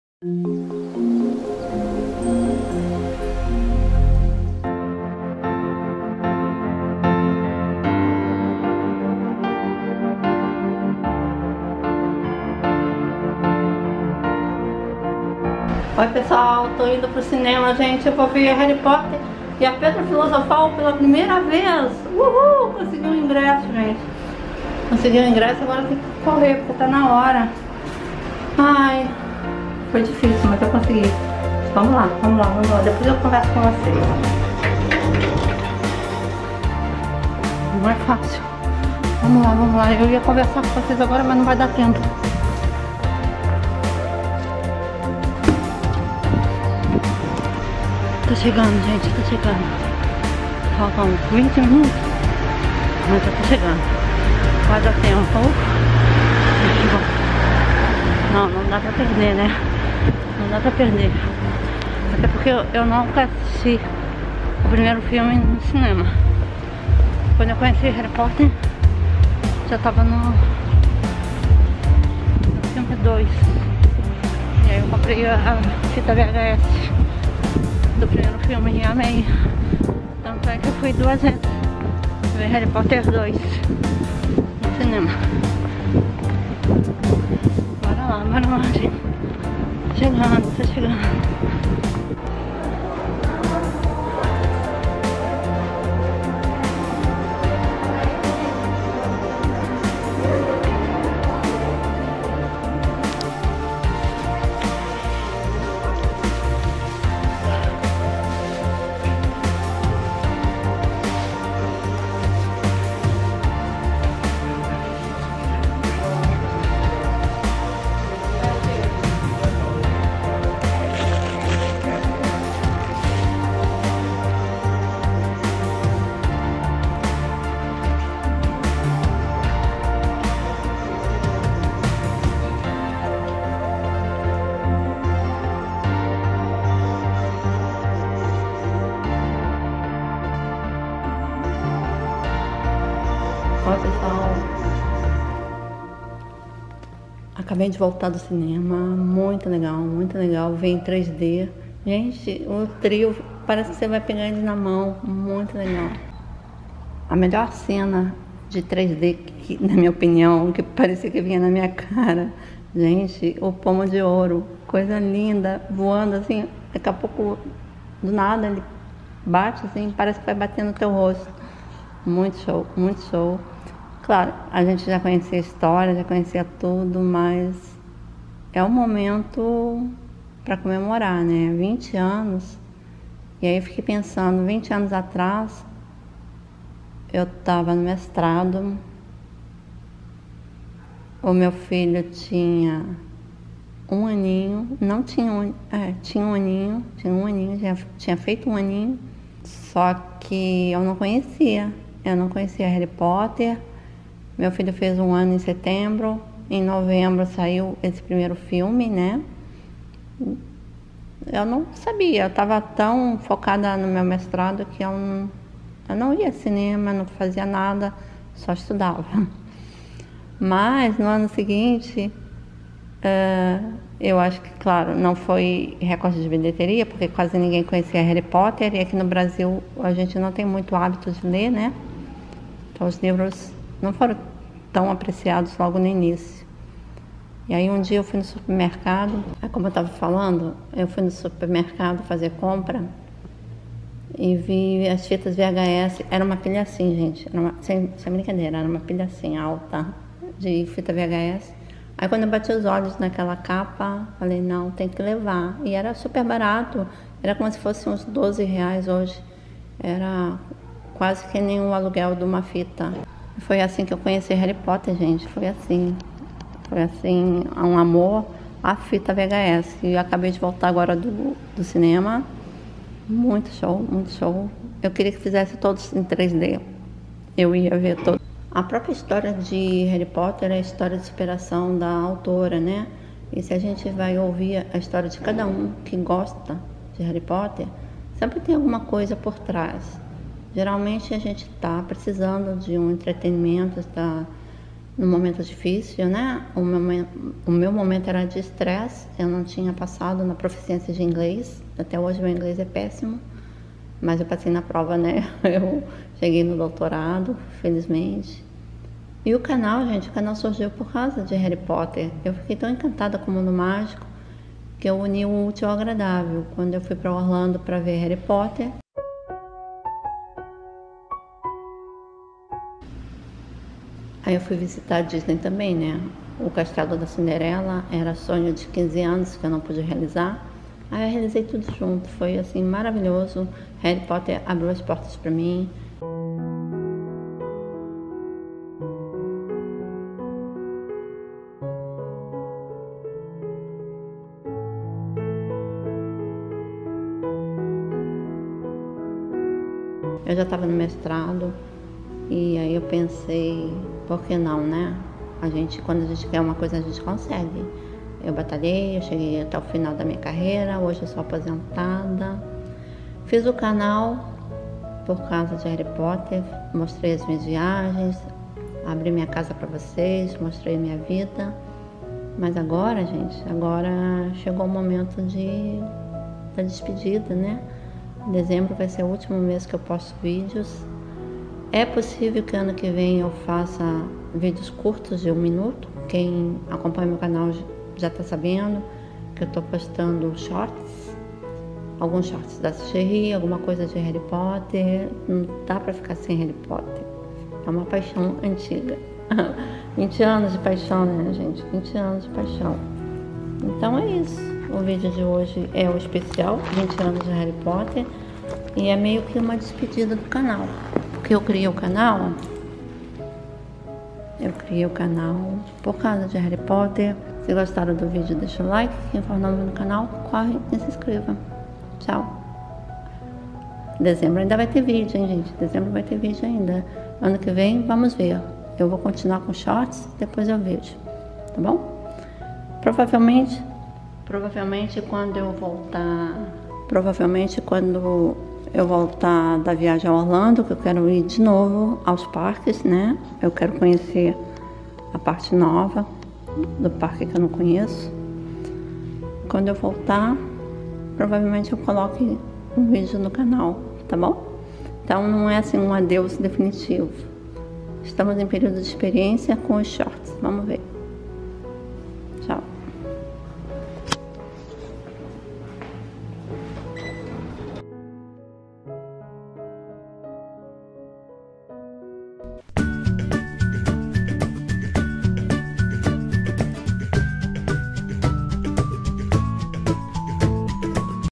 Especial de 20 anos de Harry Potter e Adeus ao canal - 🥂20 anos 🧡💚💛💙
Na sexta-feira eu fiquei sabendo que na minha cidade também iria passar o primeiro filme de Harry Potter. Fiquei muito feliz, porque não contava mais com isso, uma vez que os ingressos em Porto Alegre estavam todos vendidos. Sábado eu fui até o cinema para comprar o ingresso, aqui não vendem on line. Foi  a minha sorte, porque já havia poucos lugares. Quem deixou para comprar ontem (dia 21/11), não conseguiu.
Fiquei muito empolgada e resolvi fazer esta edição extra, gravando este vídeo! O lado ruim é que finalmente consegui falar o que há muito tempo eu evitava...  O canal está acabando... Ao menos neste formato. Estou fazendo um teste com shorts e ano que vem serão vídeos curtos de 1 minuto somente. Não tenho muita esperança que o canal vá em frente, por isso já começo a me despedir de vocês. Dezembro ainda postarei semanalmente, e terminarei com a Retrospectiva do ano.  Depois vamos ver... De qualquer forma deixo registrado aqui o meu agradecimento de todo o carinho que recebi e as amizades que fiz virtualmente.